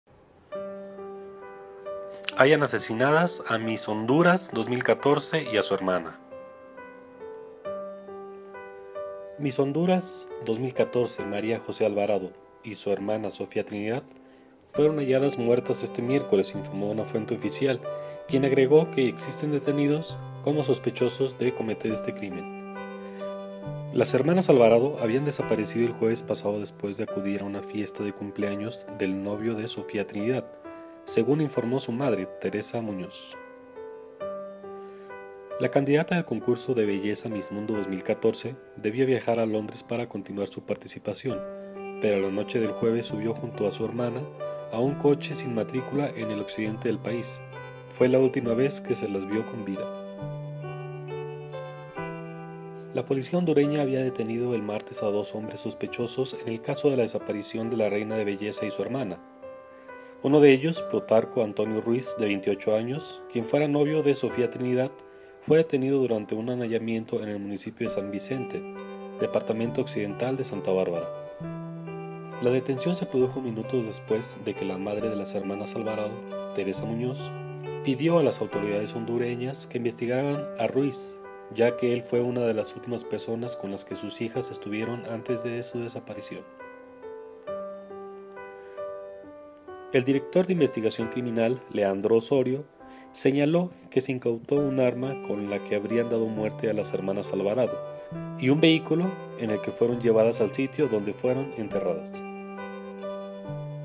MUERE MISS HONDURAS Y SU HERMANA ULTIMA NOTICIA
DESPUES DE HABER DESAPARECIDO EL PASADO JUEVES 13 DE NOVIEMBRE, FUERON ENCONTRADAS SIN VIDA ESTE MIERCOLES 19 DE NOVIEMBRE DE 2014